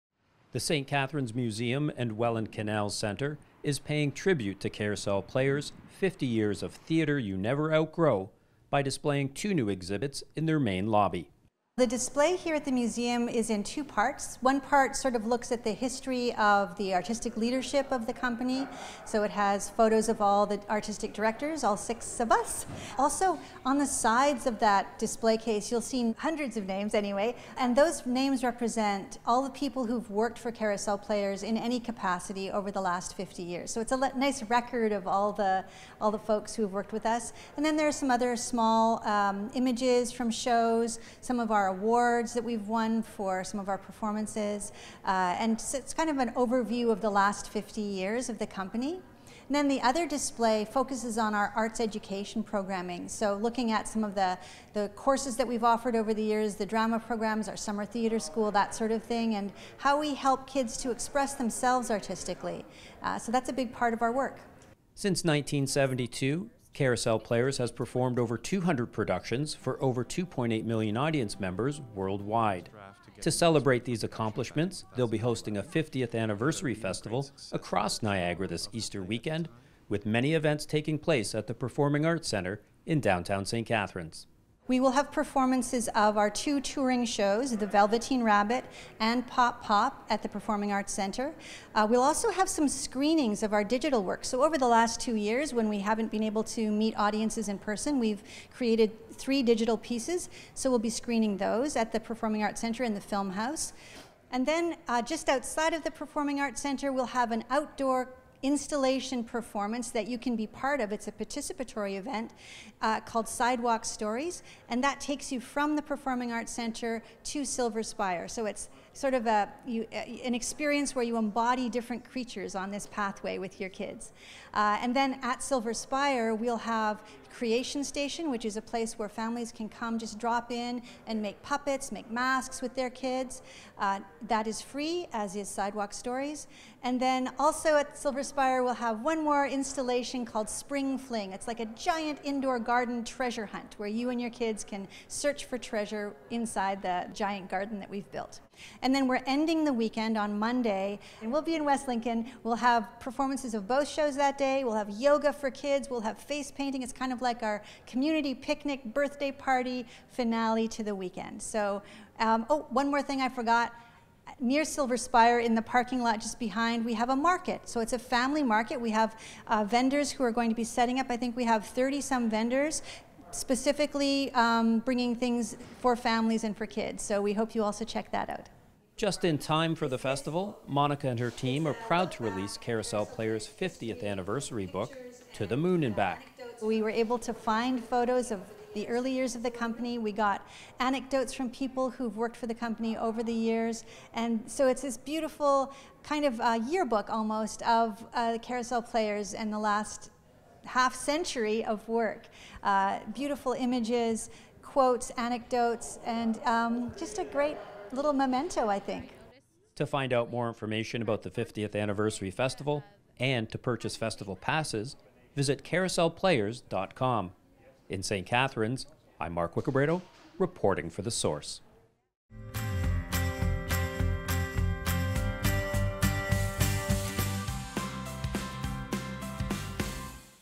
Carousel Players - St. Catharines Museum Display
Two new displays at the St. Catharines Museum and Welland Canals Centre honour Carousel Players and 50 Years of Theatre You Never Outgrow. Artistic Director Monica Dufault discusses the exhibits and talks about the 50th Anniversary Festival taking place throughout Niagara this Easter Weekend.